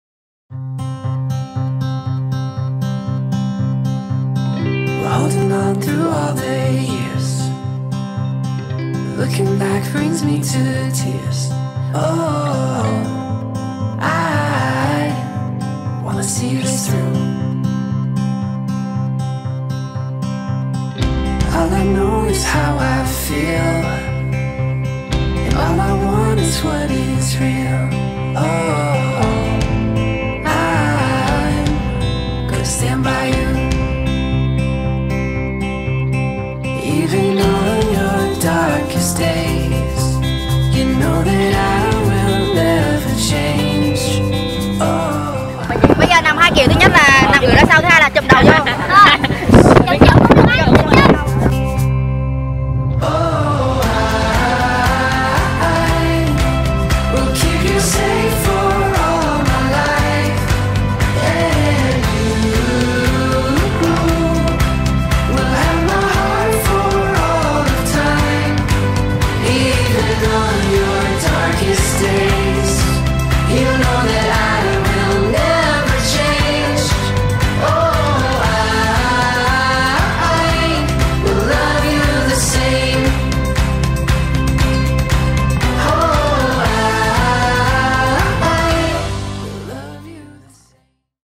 Flycam Mũi Yến - Điểm cắm trại đẹp nhất Việt Nam
Một trong những địa điểm cắm trại đẹp nhất Việt Nam, mong mọi người dù đến dù đi cũng xin đừng để lại rác chốn này.